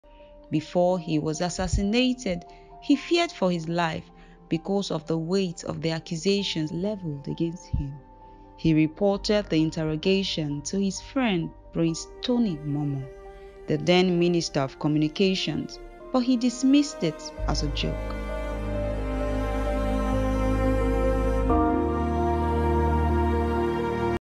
How Dele Giwa was Assassinated
Before he was assassinated , he feared for his life because of the weight of the accusations leveled against him, he reported the interrogations to his friend Prince Tony Momoh, The then Minister of Communication but he dismissed it as a joke